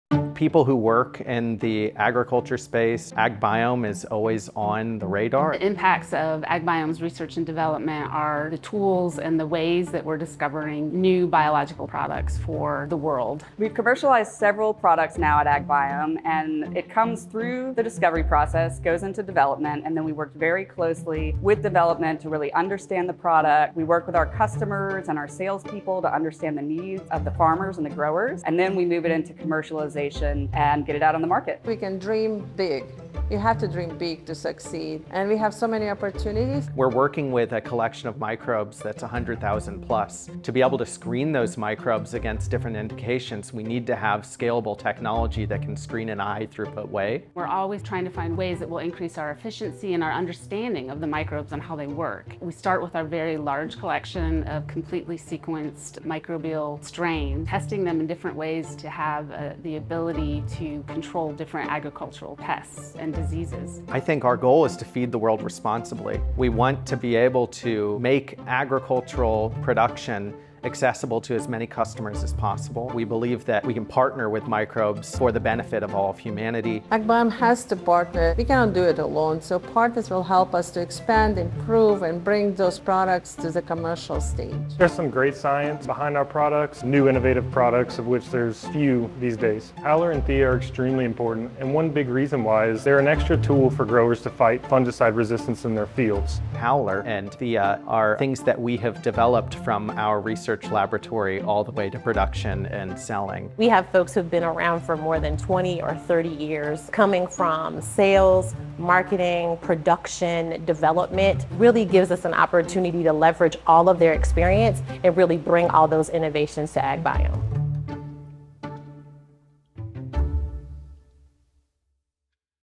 We Are AgBiome - Transforming Agriculture Through Innovation and Technology
One mission: To harness the power of the planet’s natural microbial communities for human benefit. One purpose: To help feed the world responsibly.

WeAreAgBiome - A community of world-class scientists and experts united in our mission to revolutionize agriculture.

Our world is changing and we’ve seen firsthand the challenges today’s growers are facing. With a team of world-class scientists and state-of-the-art facilities, we're leading the way in agricultural innovation, developing new products and technologies that address the needs of growers today and our changing environment. All while helping them achieve higher yields, healthier crops, and more sustainable practices.

Together, we are changing the landscape for growers and the planet. Together, we are changing the future of agriculture.